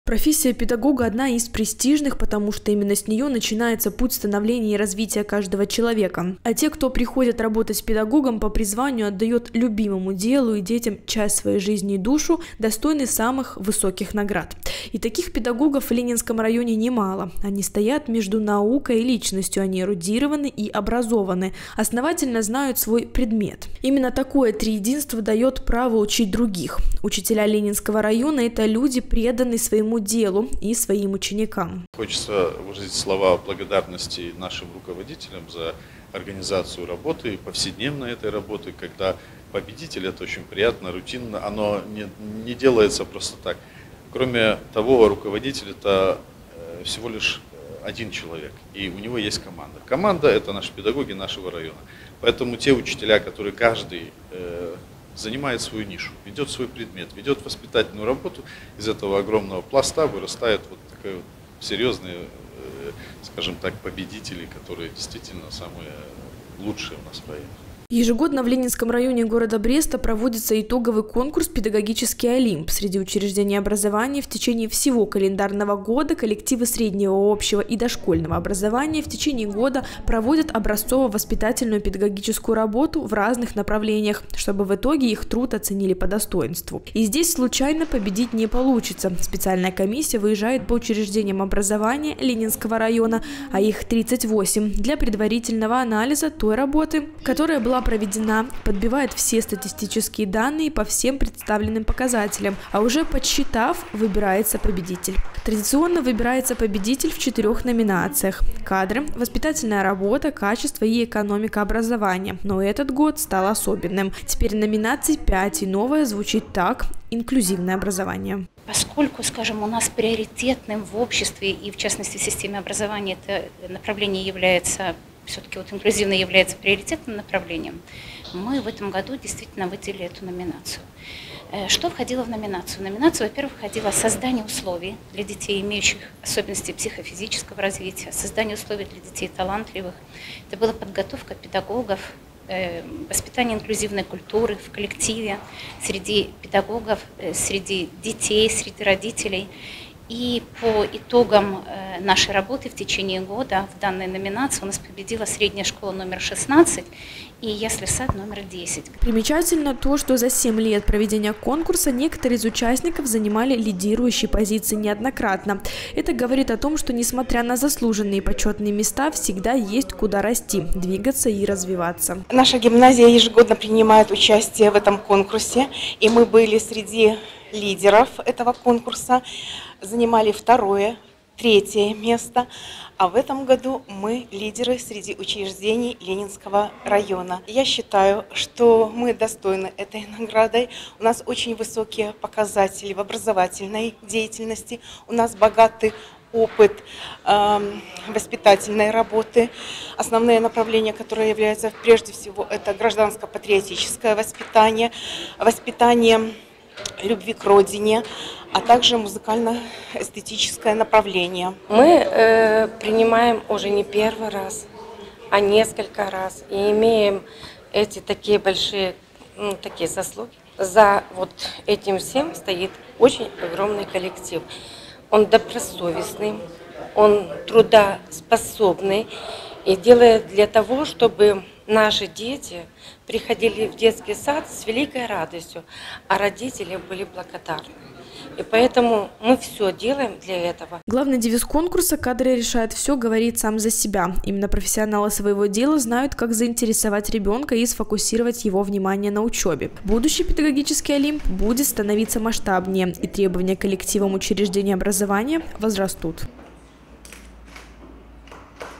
2023-03-21 г. Брест. Подведение итогов конкурса «Педагогический Олимп». Новости на Буг-ТВ.
Идейные вдохновители, лидеры, новаторы, те, кто сеет вечное и разумное – именно так можно описать наших брестских педагогов. Ежедневно они закладывают в детях не только знания и навыки, но и многие принципы и черты характера. Словом, тот код, расшифровав который, ребёнок становится полноценной и развитой личностью. Воспитатели и учителя Ленинского района Бреста, чьи учащиеся – призёры олимпиад и победители различных конкурсов и проектов, получили свои награды за вклад в развитие образования и, конечно, воспитание подрастающего поколения. «Педагогический Олимп» - ежегодный районный конкурс. В Ленинском районе города Бреста он проводится с 2016 года с целью повышения эффективности работы образовательного учреждения. 2023 год не стал исключением.   Прямо сейчас я расскажу о победителях – тех, кому присвоен почётный статус – лучшее учреждение образования Ленинского района Бреста.